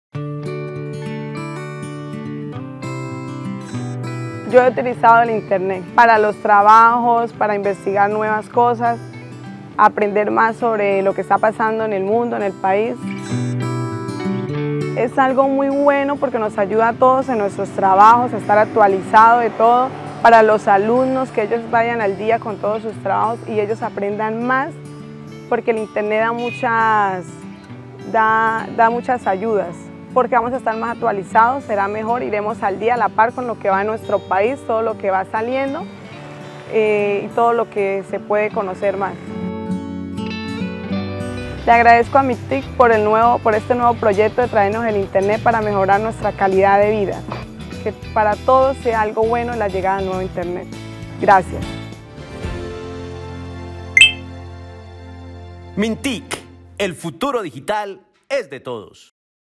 Yudi Pico, del corregimiento La Esmeralda en Arauquita, Arauca
LeCumplimosAColombia llevando conectividad🌐 al corregimiento La Esmeralda en Arauquita, Arauca, donde Yudi Pico nos cuenta cómo ahora sus amigos y familiares están aprovechando el internet para realizar actividades académicas 📚.